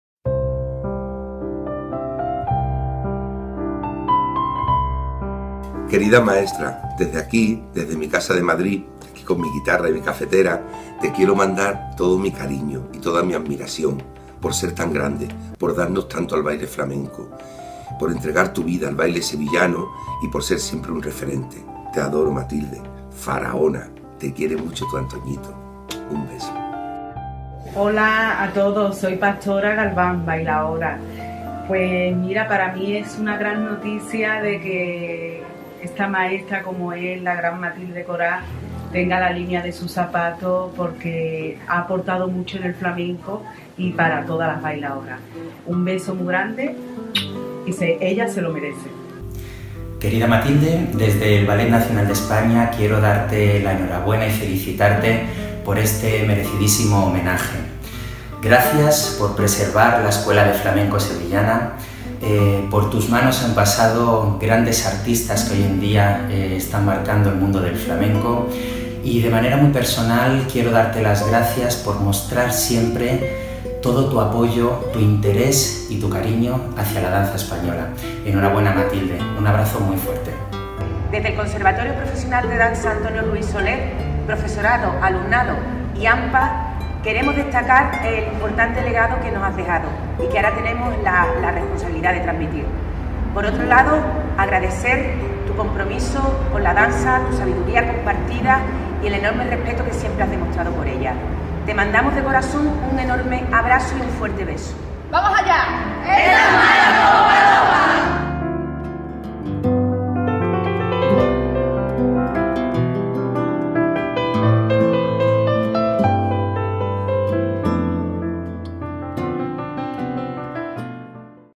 Saludos de famosos en la gala Matilde Coral
El 9 de diciembre de 2016 se presentó en una gala en el Auditorio Box Sevilla la colección de zapatos Matilde Coral. El bailaor Antonio Canales y el director del Ballet Nacional, entre otros, le enviaron estos afectuosos saludos.